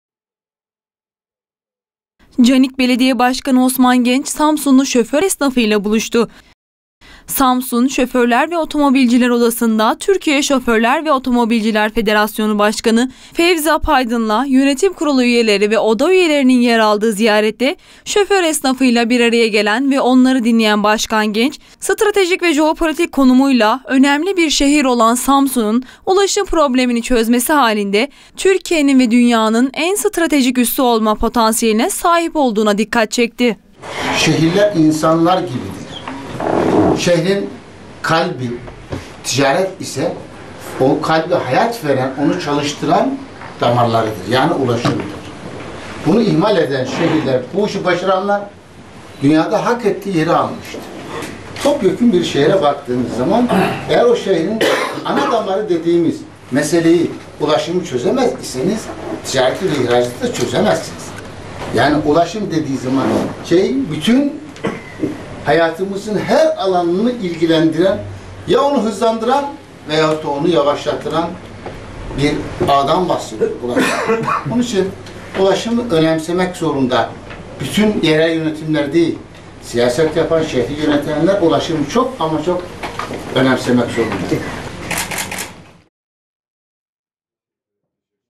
Ulaşım sorunu çözülmeli
Canik Belediye Başkanı Osman Genç, "Şehirlerin yarıştığı yeni bin yılda Samsun'u yarışa dahil etmek istiyorsak mutlaka ulaşım sorunlarını çözmeliyiz. Ulaşım, bir şehrin hayat damarıdır" dedi